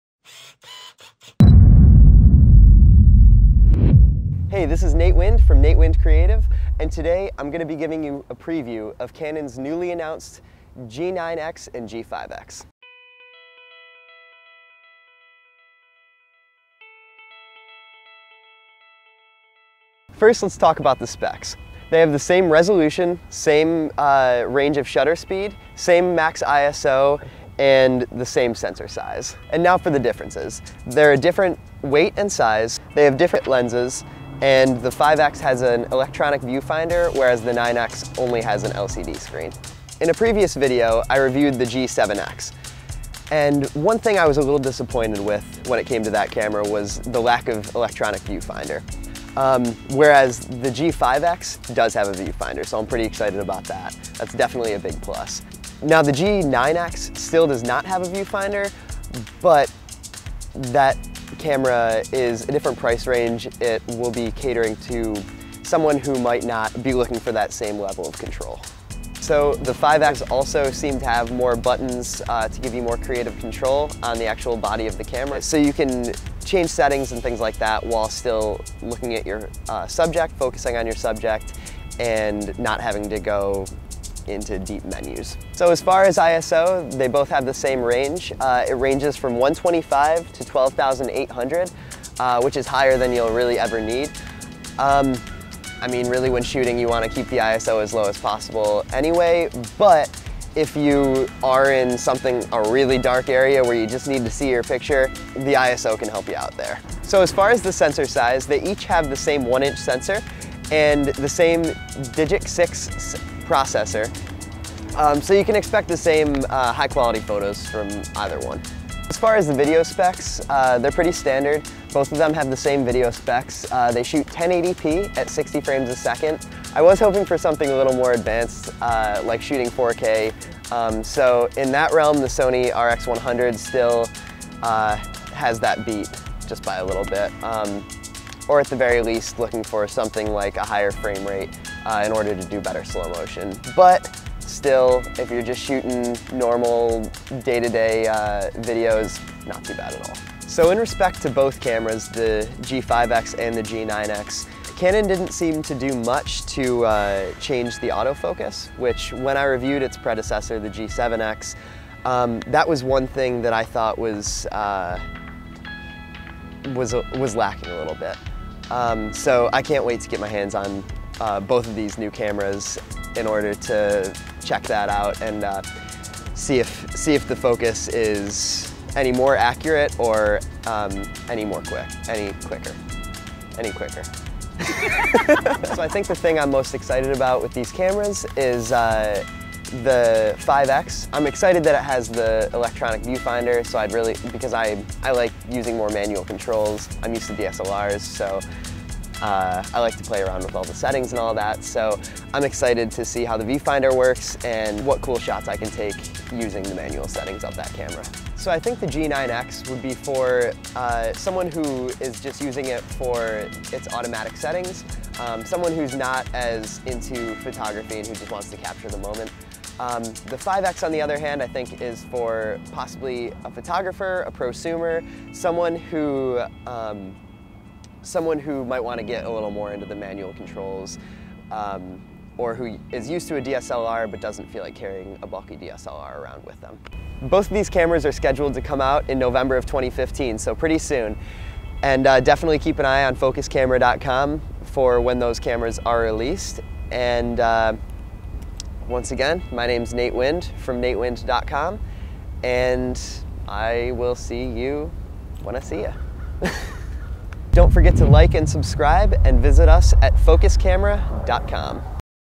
Canon G5X & G9X Overview
Nate Wind shares with us his initial thoughts on the Canon G5 X and G9 X